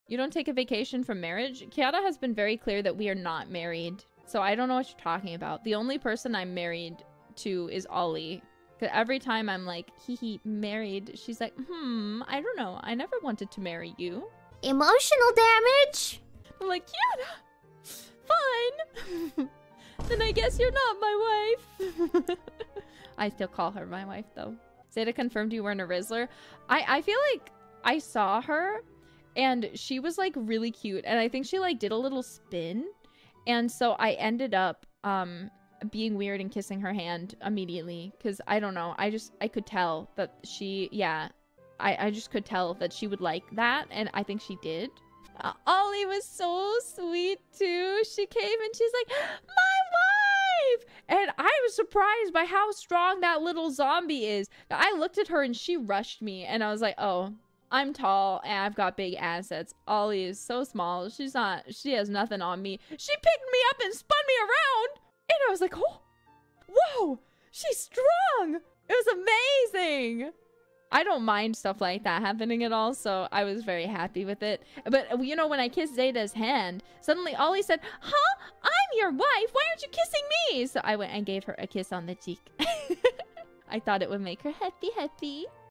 😲 Nerissa: Ollie Is My Wife... Kiara Be Like: !!!【Hololive EN】
Sausage:
Let's talk about Japan!【Chatting】
Credit:
DDOLBANG11